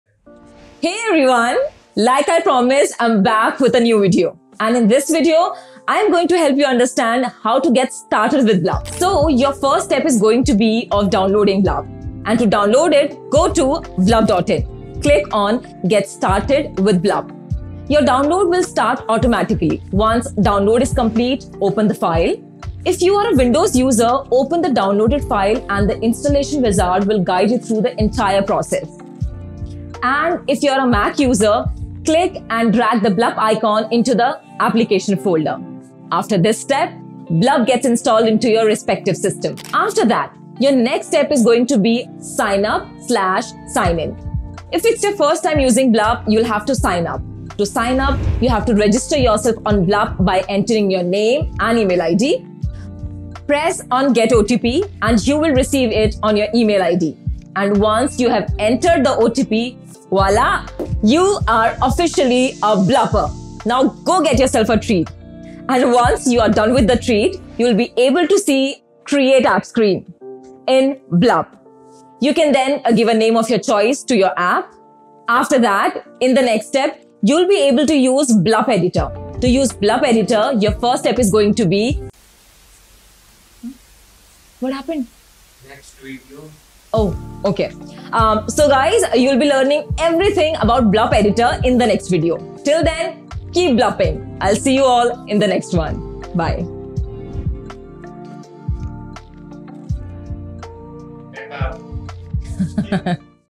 Getting started with Blup. Learn how to build mobile apps using Blup!
Blup is an app development tool that helps you create Flutter apps graphically and also hassle free. So to learn how to create these apps, you need to first learn about Blup. And in this video we are guiding you on "How to get started with Blup".  So, now to get started with Blup,

-Your first step is going to of Downloading Blup and to download it,
- Go to blup.in & click on get started with Blup. Your download will start automatically.
- Once download is complete, open the file.
- Windows users, open the downloaded file and the installation wizard will guide you through the entire process
- Mac users, click and drag the Blup icon into the applications folder.
- Then Blup gets installed into your respective system.
-Then your next step is going to be Sign Up/ Sign In. If its your first time using Blup. You’ll have to signup.
- Enter OTP
- Now you’ll be able to see a Create App Screen in Blup. You can then give a name of your choice to your app.
- Now in the next step you’ll be ble able to use Blup Editor.

To learn about Blup Editor, wait for the next video where we'll give you a complete tutorial about Blup Editor.